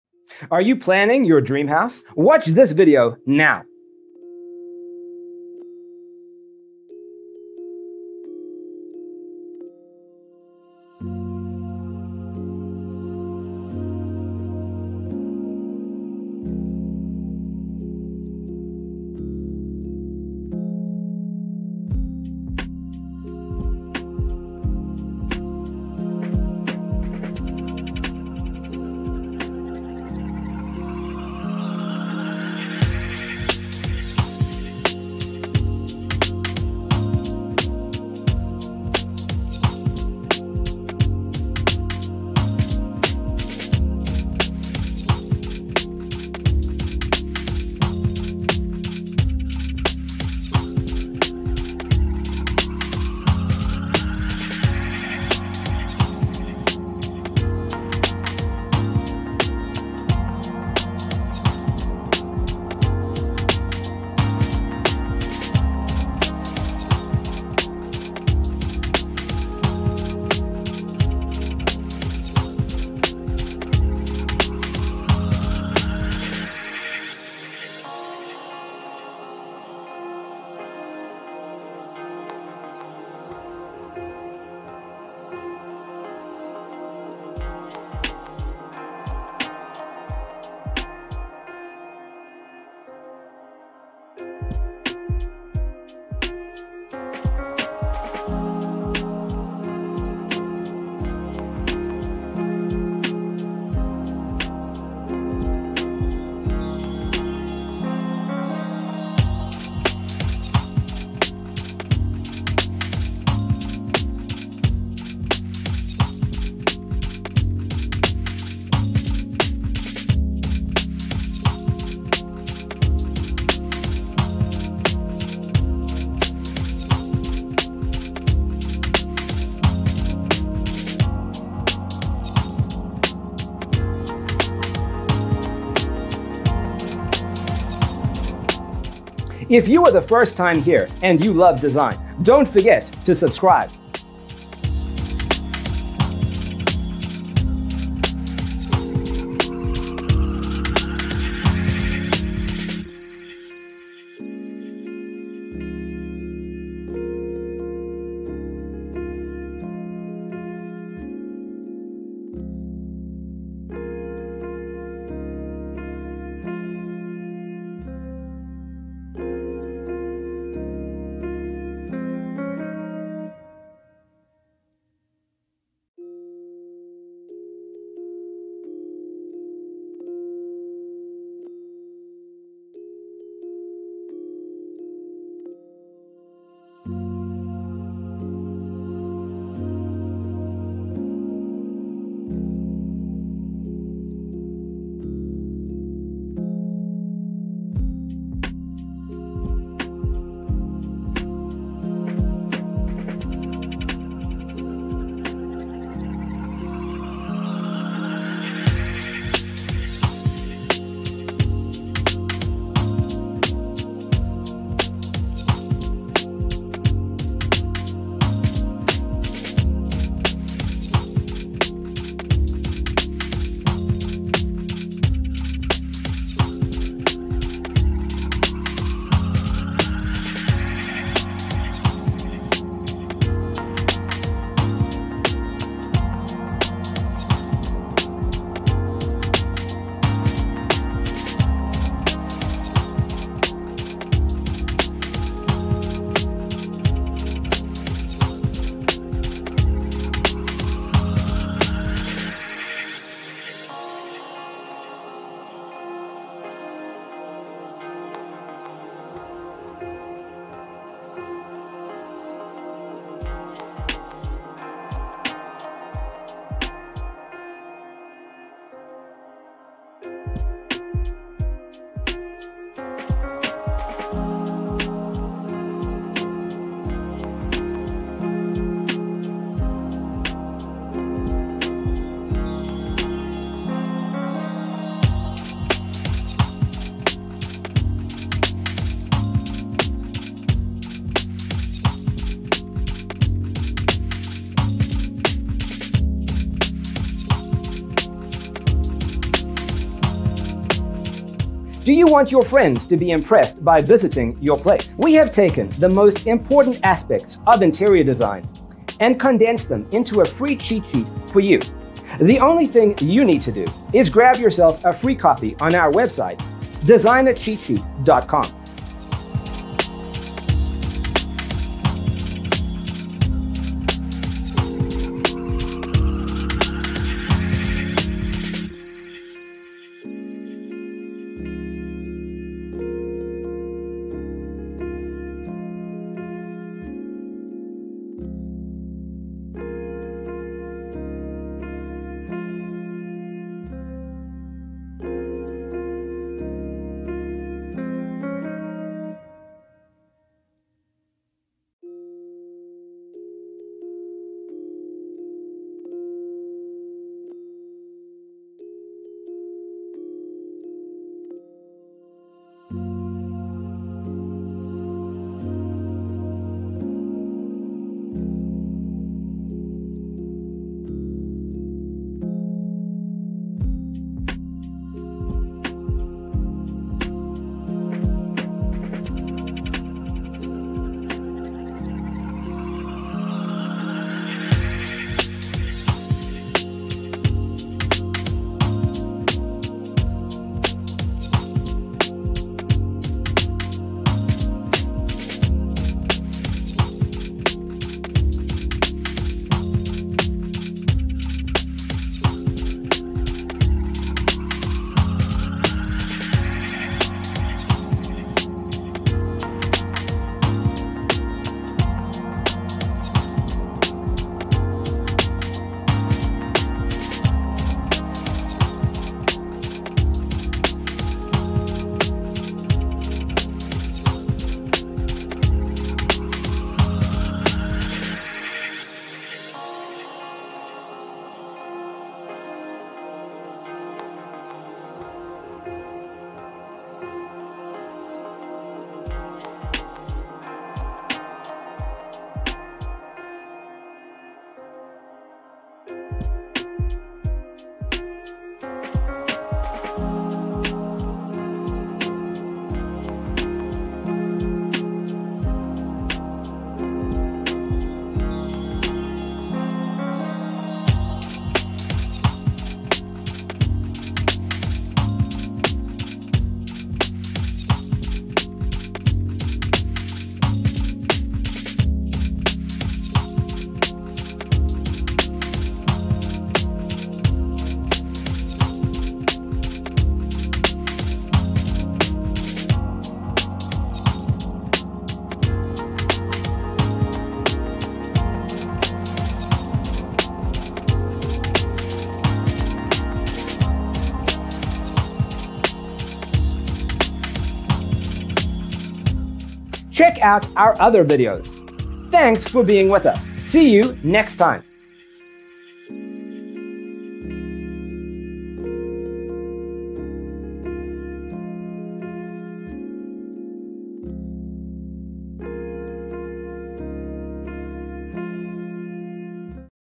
75 Coastal Wine Cellar Design Ideas You'll Love 😊
Browse photos of coastal home wine cellar designs and decor. Discover ideas for custom beach style wine rooms and closets, including inspiring nautical wine cellar layouts and storage solutions.

0:00:00 - Intro
0:00:12 - Coastal Wine Cellar Design Ideas You'll Love: Inspirations 1 to 25
0:03:03 - Coastal Wine Cellar Design Ideas You'll Love: Inspirations 26 to 50
0:06:07 - Coastal Wine Cellar Design Ideas You'll Love: Inspirations 51 to 75